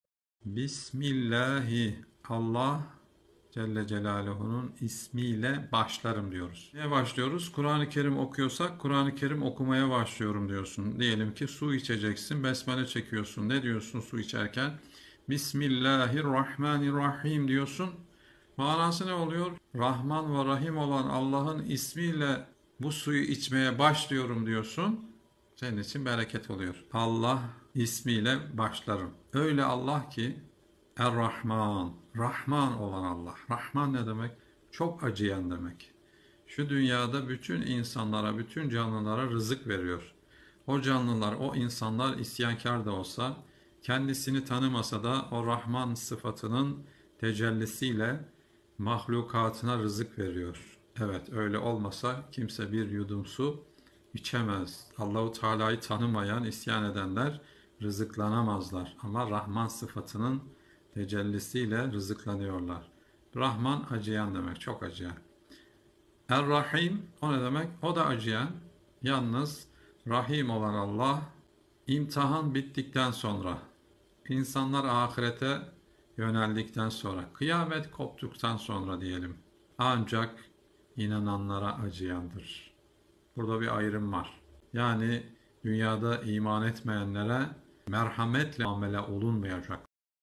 Besmelenin Mânâsı
@selamihocam kanalımızdaki derslerimizden oynatma listesine girerek istifade edebilirsiniz.

Rahmân  ve  Rahîm  olan  Allâhın  İsmiyle  başlarım..

Rahmân  demek   Dünyada  Mü min , Kâfir  bütün kullara  Hakiki  nimet  veren  ve  çok acıyan..
Rahim  demek  Âhirette  sadece  Mü min  kullarına  Acıyan ve Rahmet  eden demektir.. #